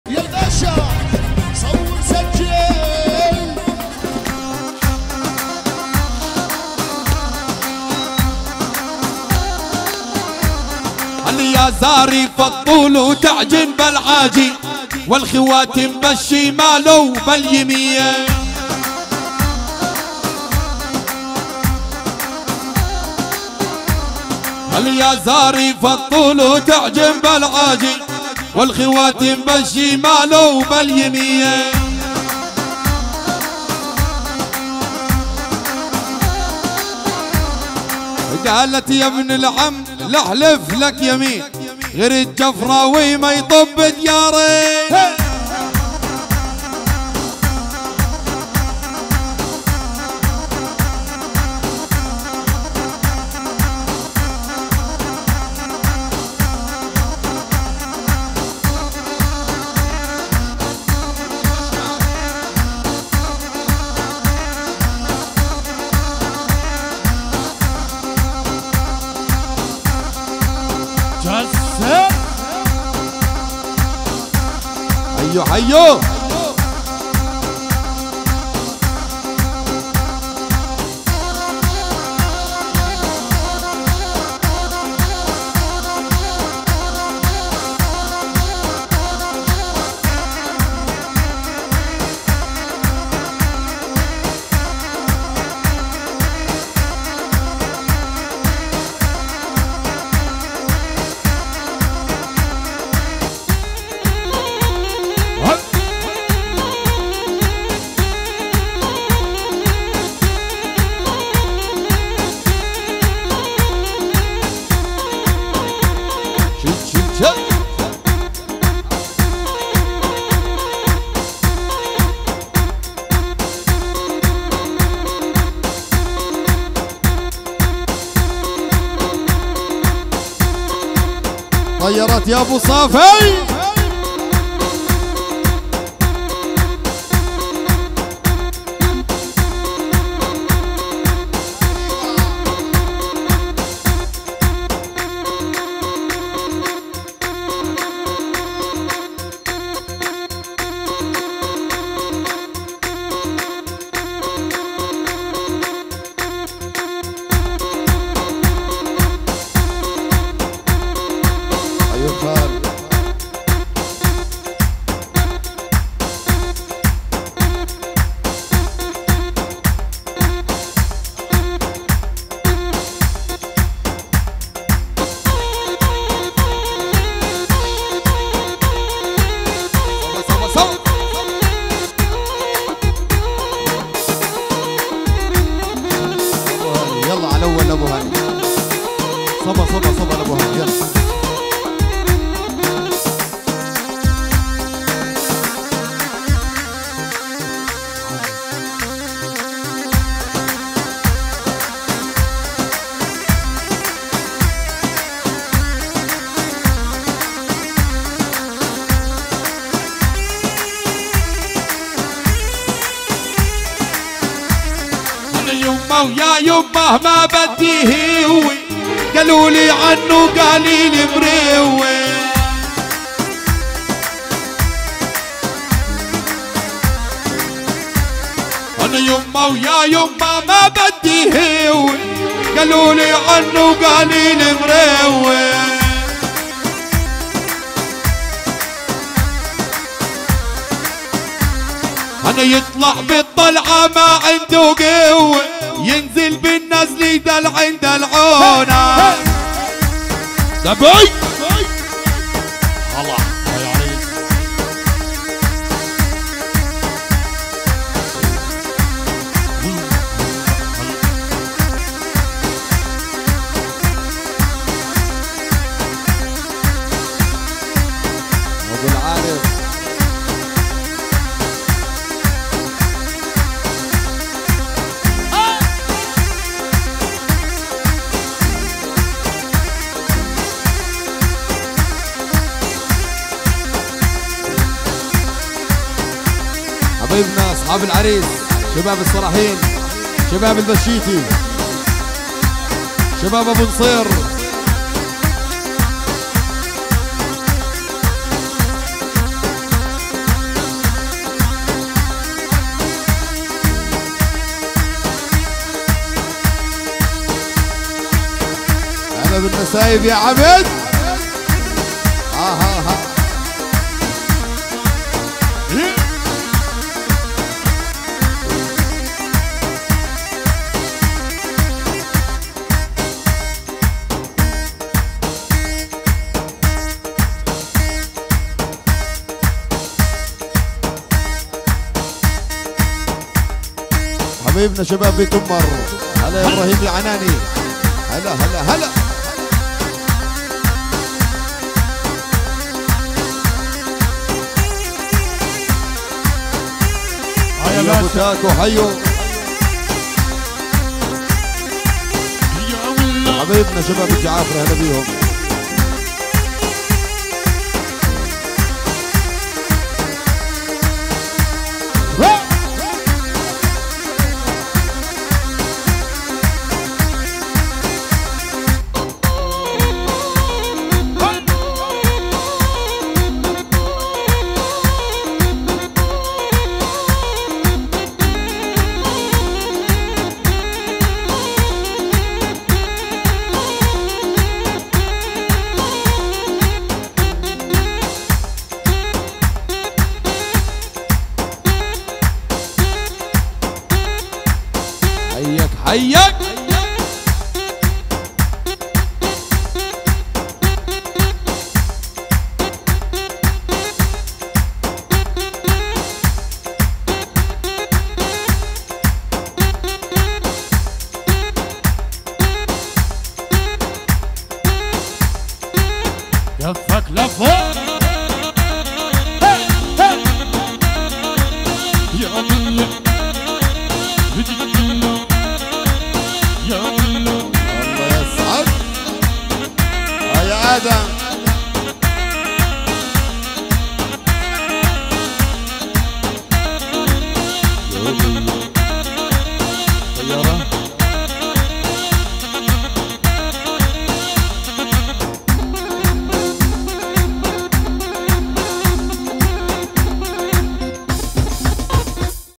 النجم فراس فراشات💥👈من اجمل وارتب الدبكات الجنوبيه الفلسطينيه💥👈مهرجان ادهم العكل👉💥تسجيلات الباشا💥💪
مع تحيات تسجيلات_الباشا

 تسجيلات_الباشا

للحجز والاستفسار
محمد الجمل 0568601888
محمد الجمل وتس اب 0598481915